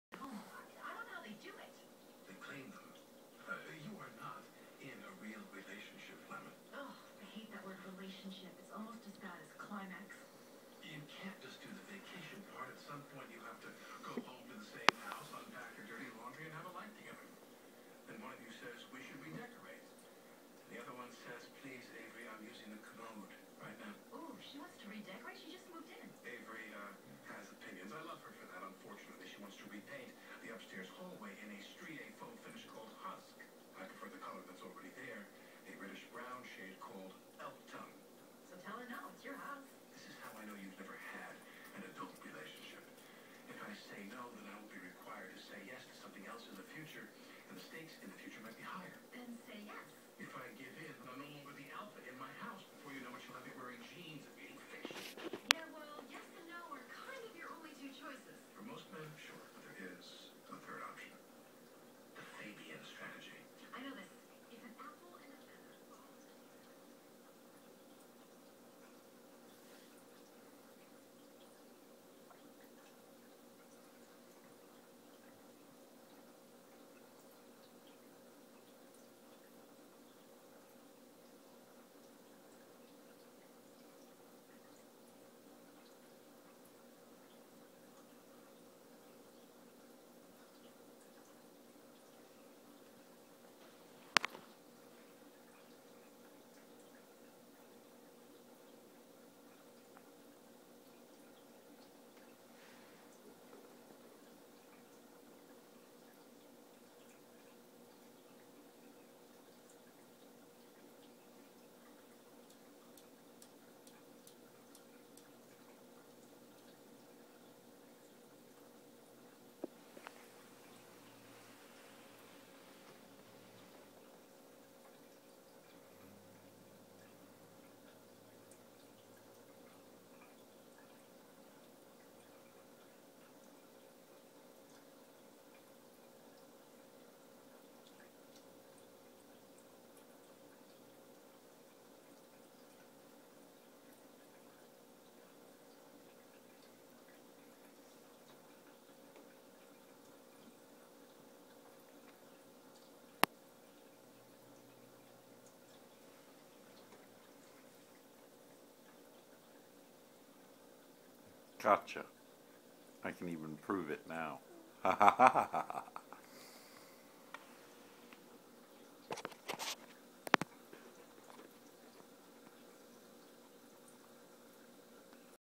The Cat and the Tank
Caught the cat on video trying to work out the complexities of glass.